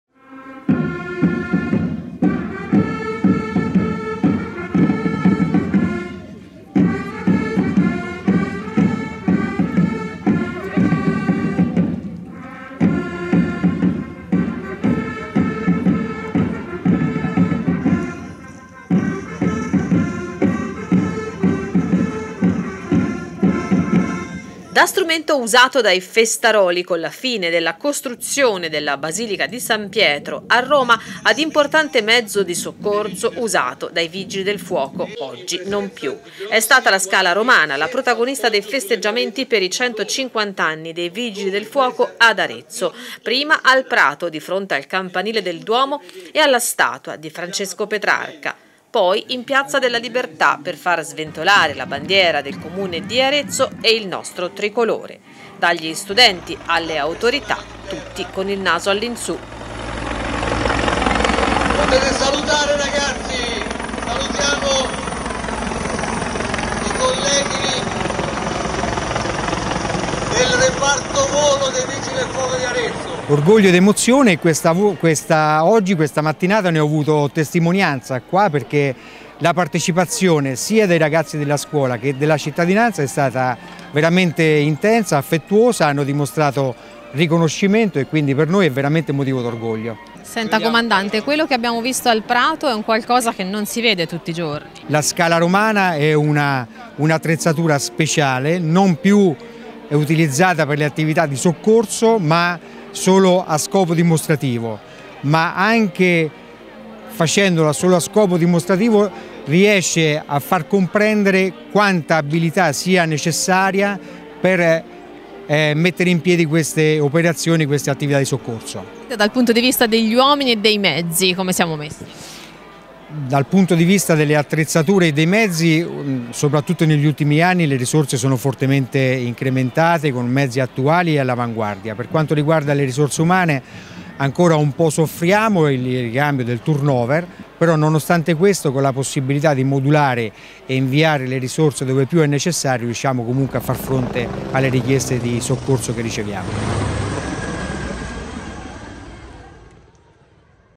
Vigili del fuoco, una storia lunga 150 anni. Al Prato di Arezzo, la scala romana
Da strumento usato dai 'festaroli' con la fine della costruzione della Basilica di San Pietro a importante mezzo di soccorso usato dai Vigili del fuoco, oggi non più.
E' stata la scala romana oggi la protagonista dei festeggiamenti per i 150 anni dei Vigili del Fuoco. Prima al Prato di fronte al campanile del Duomo e alla statua di Francesco Petrarca, poi in piazza della Libertà per far sventolare la bandiera del comune di Arezzo e il nostro tricolore.
Dagli studenti alle autorità tutti con il naso all'insù.
Un momento anche per fare il punto su uomini e mezzi. “Sotto il profilo dei mezzi – ha detto il comandante di Arezzo Fabrizio Baglioni – le risorse sono fortemente incrementate con dotazioni attuali e all'avanguardia. Per quanto riguarda gli uomini, siamo sempre un po' in sofferenza per il turn over, ma con la possibilità di inviare le risorse dove più è necessario riusciamo comunque a far fronte alle richieste di soccorso che riceviamo”.